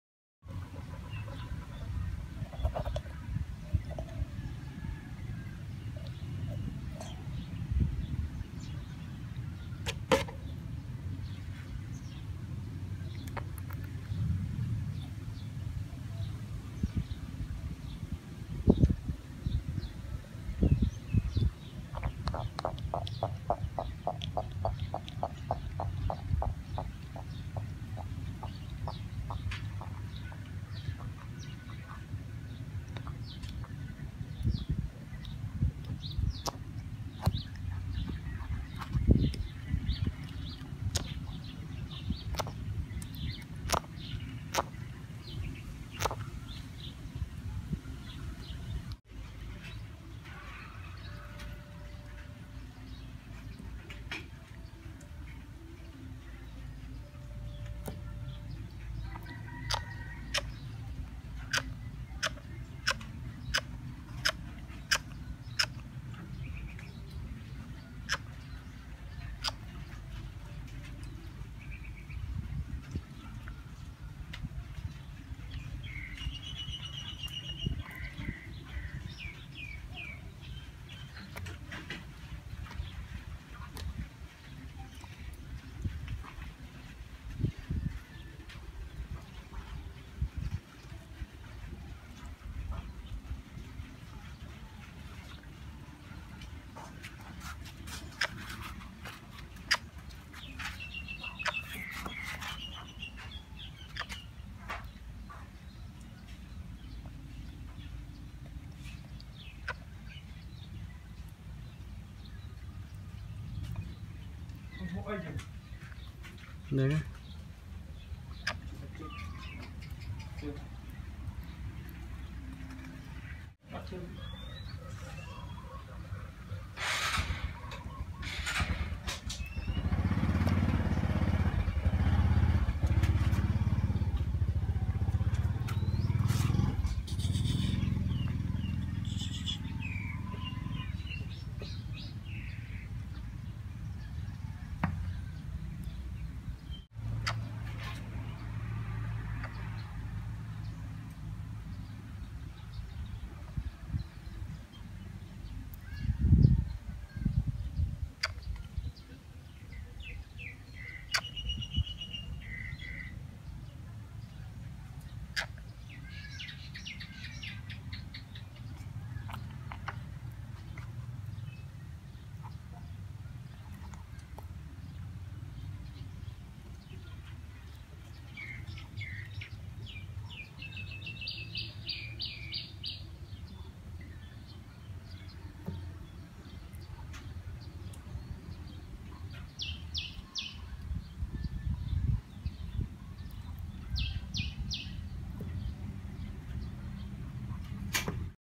What It’s fish?
Hello all my belove visitor.this video i would like love to show you about my technique cooking in my country| Cambodia.if all of you support my videos please click subscribe comment and like my channel.